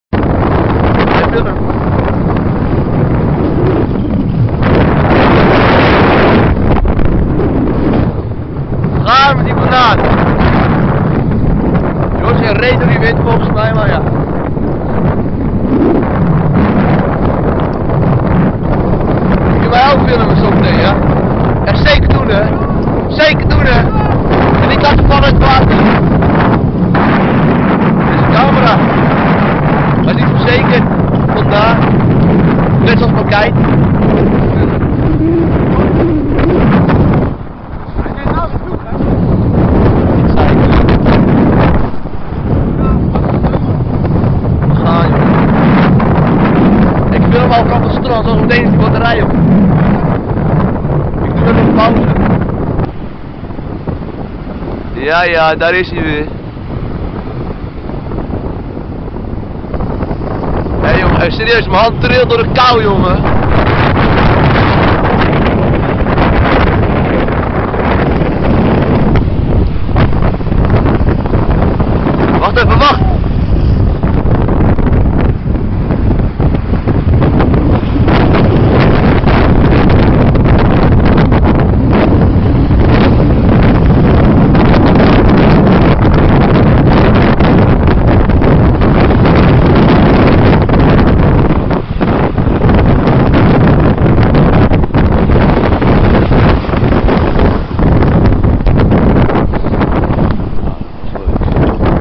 Kite Egmond Aan Zee - Water temp: 4 graden
koud. brrrrr daggie @ egmond aan zee